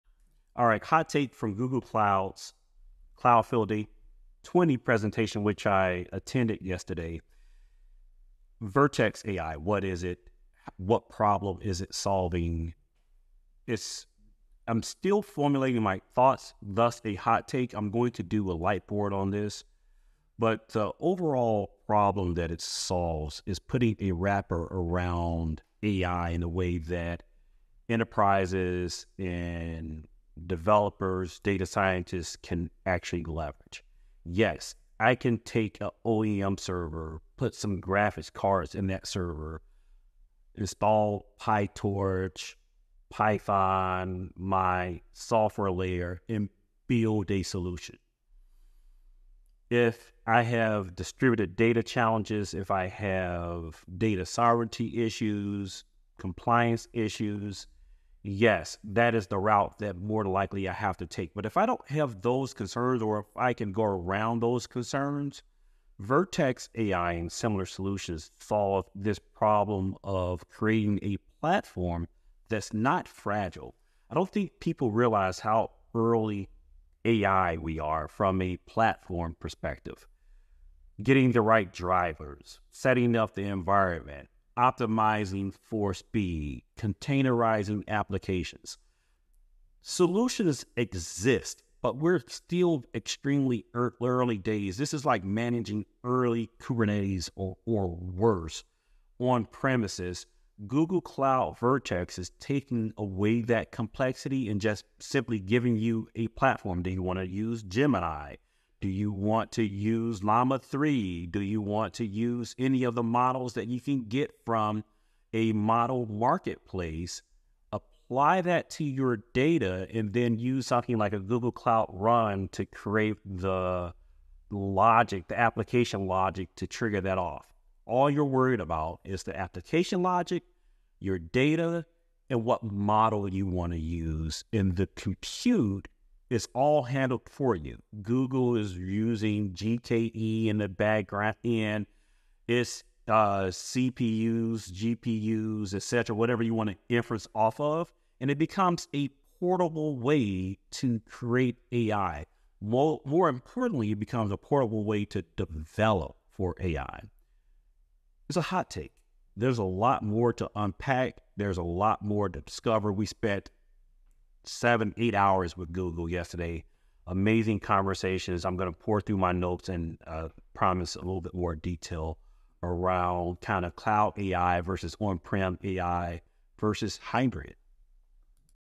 Unlocking AI Potential: Insights into Google Cloud's Vertex AI Platform
In this session, we dive deep into Google Cloud's latest offering, the Vertex AI platform. Our speaker provides an in-depth overview of Vertex AI, highlighting its functionalities, the problems it aims to solve, and its advantages over traditional AI development methods. Learn how Vertex AI simplifies complex AI processes, making it easier for developers, data scientists, and enterprises to implement AI solutions without the hassle of setting up the environment.

Key Topics Covered:
- Overview of Google's Vertex AI platform
- The problem Vertex AI is designed to solve
- Advantages of using Vertex AI over traditional methods
- The early stage of AI platform development
- The potential of Vertex AI in simplifying AI development and application

Key Takeaways:
1. **Simplification of Complex AI Processes**: Vertex AI provides an efficient solution, enabling users to focus on application logic, data, and models without worrying about the backend complexities.
2. **Advantages for AI Development**: The platform leverages Google Kubernetes Engine, ensuring a portable and robust AI development environment.
3. **Early Stages and Future Potential**: While AI platforms are still in their infancy, Vertex AI promises to reduce setup complexities, paving the way for easier AI integration.

Quotes:
- "Vertex AI solves the problem of creating a platform that's not fragile."
- "All you're worried about is the application logic, your data, and what model you want to use."
- "Google Cloud Vertex is taking away that complexity and simply giving you a platform."

Join us as we explore how Vertex AI is revolutionizing the AI landscape and what it means for the future of AI development.